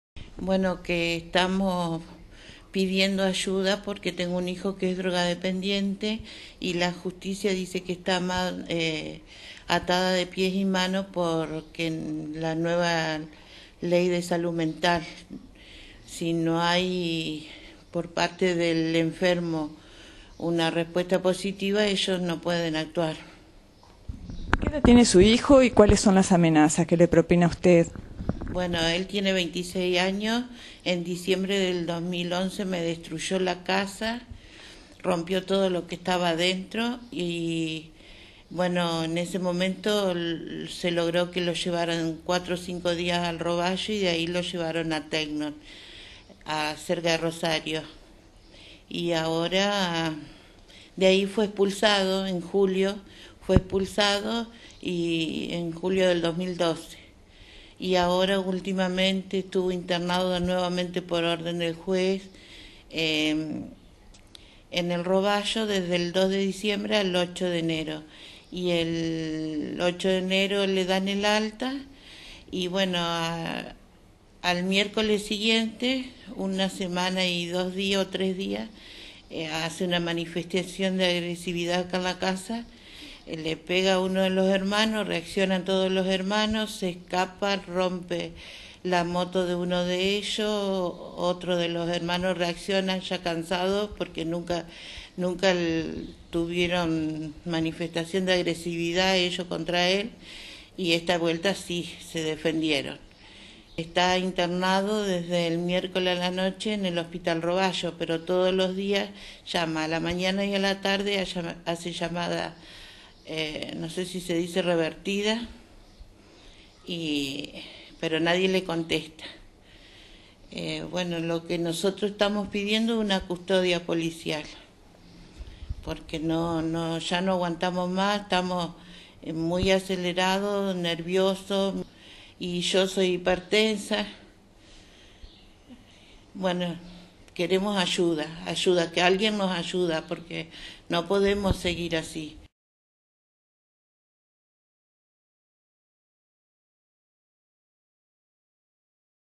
Una madre realiza pedido desesperado
Gladys Flematti tiene un hijo drogadependiente y violento. La mujer pide custodia para proteger a su familia.